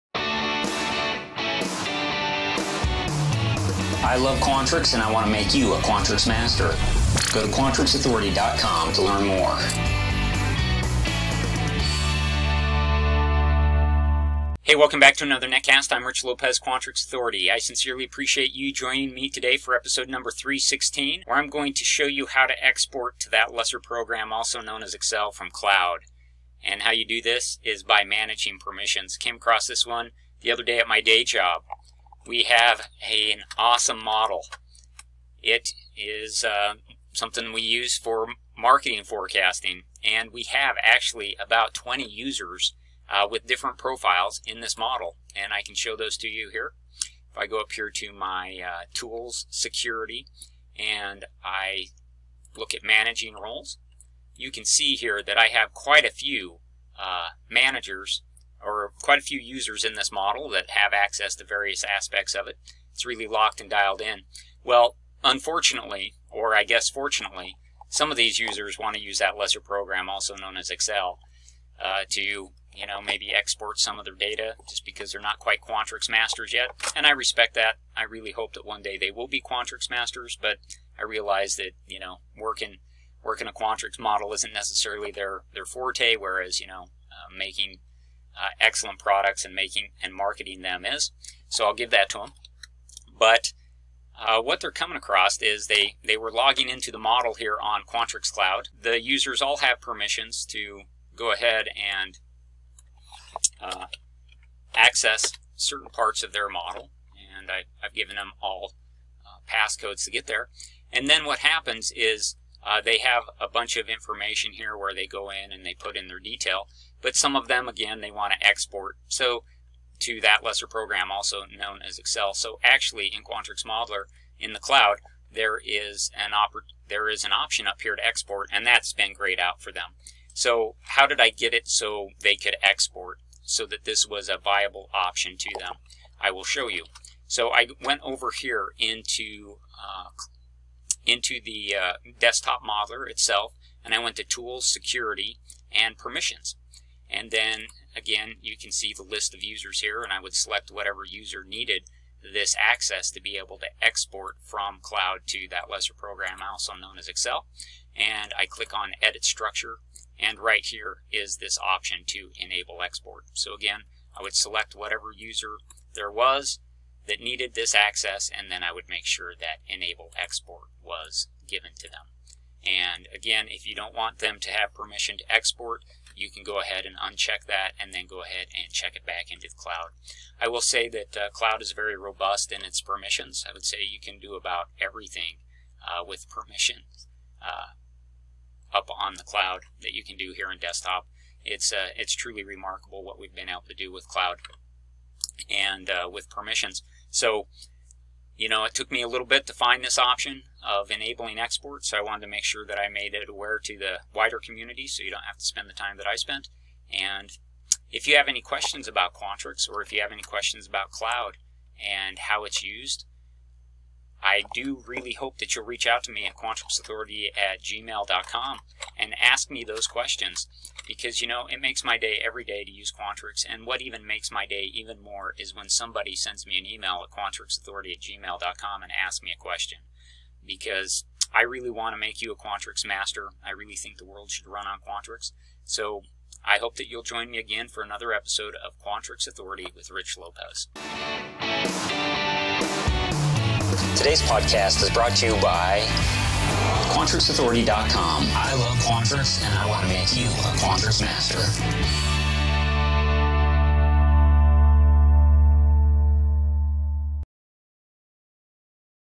Learn Quantrix Modeler: How to Export to Excel from Qloud - Permissions 0316
How to Export to Excel from Qloud - Permissions 0316

Where are the permissions located to export to Excel from Quantrix Qloud.
Where are the permissions located to export from Qloud to Excel.

How to manage user profiles in Quantrix.  How to use Quantrix Modeler to allow multiple user edits or editing.